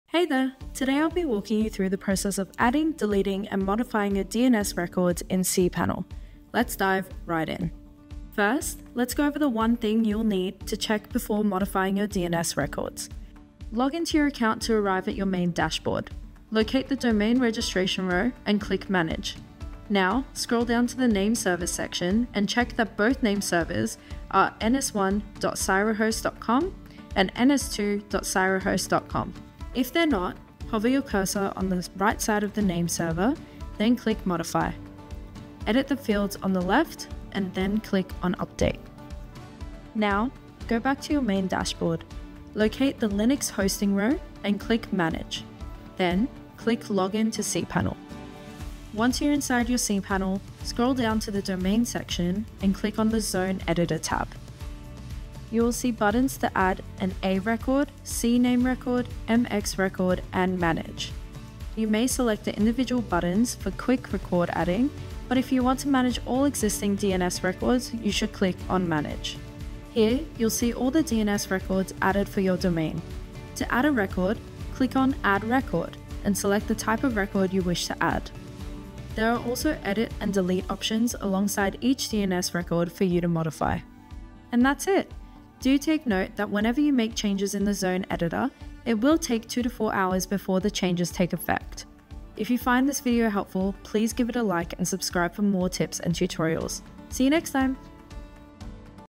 Manage DNS Records in cPanel Easily | Add, Edit & Delete A, CNAME, MX Records
Need to update your DNS records? This easy-to-follow tutorial walks you through everything you need to know to manage DNS records in cPanel.

We’ll cover how to check your domain’s name servers, access the cPanel Zone Editor, and add, edit, or remove A, CNAME, and MX records. Whether you’re setting up email, pointing your domain, or troubleshooting, this guide has you covered.

🔹 Perfect for beginners and web hosting users using Syrahost or similar services.
🔹 Make sure your name servers are correctly set to ns1.syrahost.com and ns2.syrahost.com before updating your DNS.

0:00 – Intro
0:09 – Check & Update Name Servers
0:41 – Access cPanel
0:50 – Open Zone Editor
1:10 – Add, Edit, or Delete Records
1:26 – DNS Propagation Time
1:32 – Outro